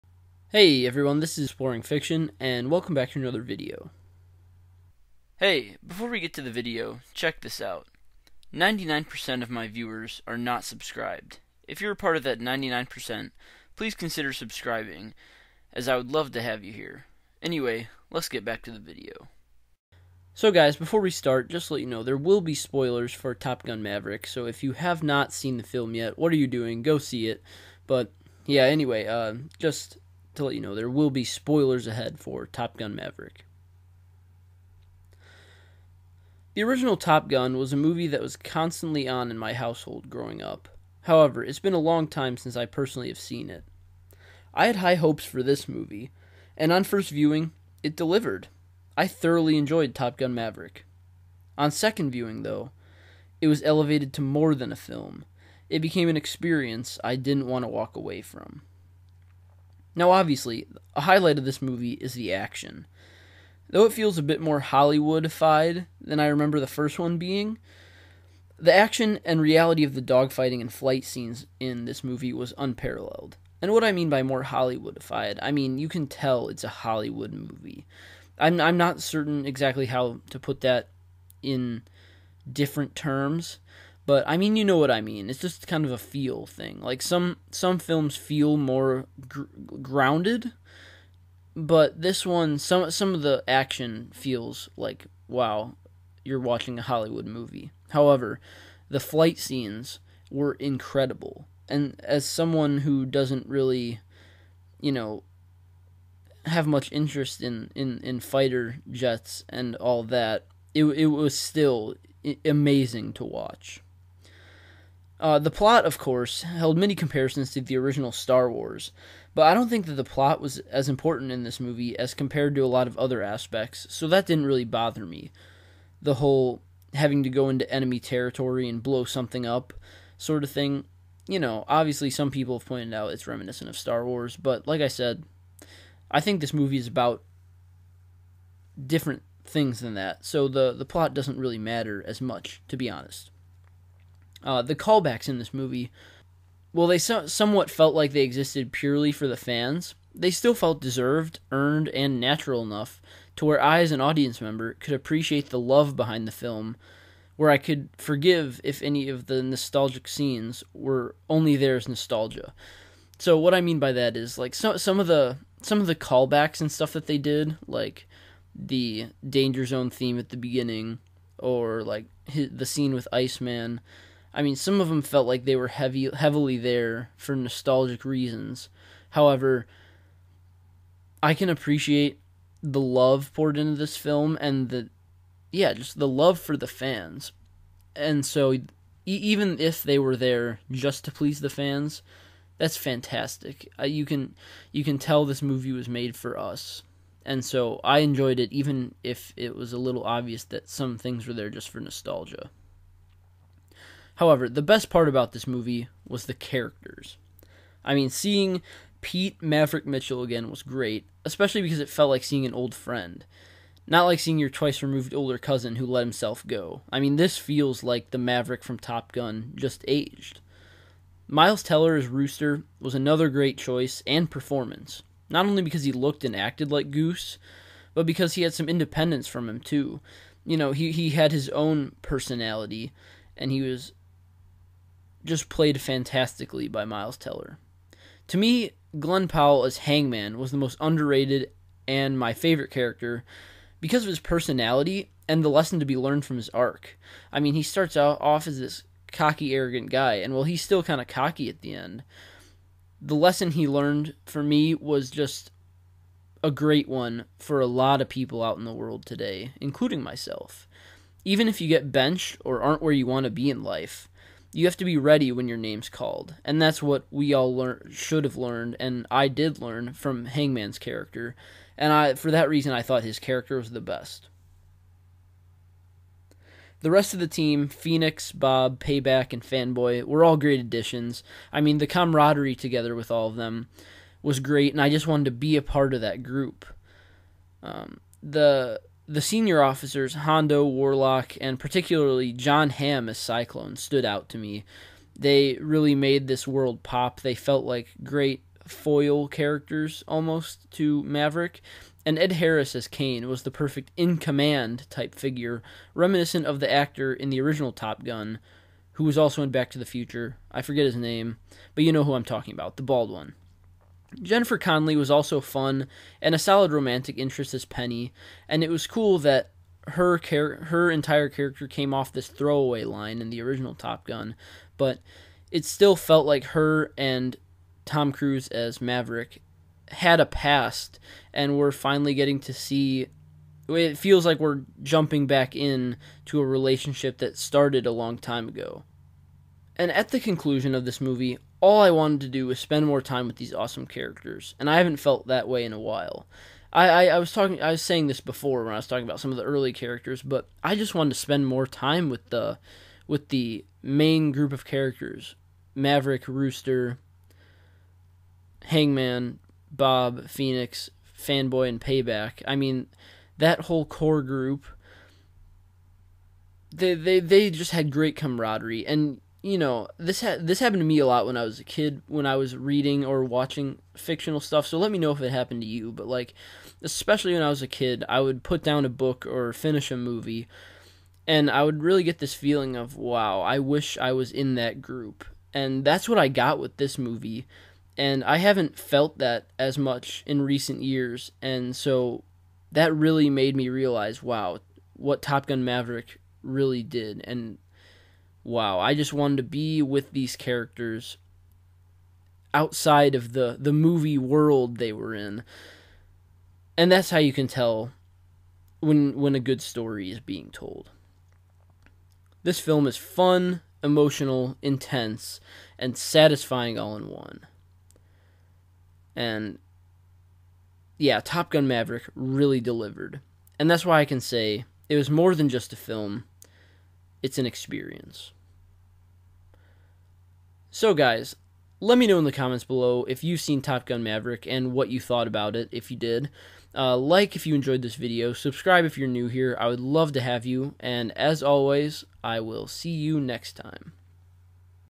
Top Gun: Maverick Was More Than a Film
In this video, I give you my thoughts on Top Gun: Maverick. Hint: it was a masterpiece!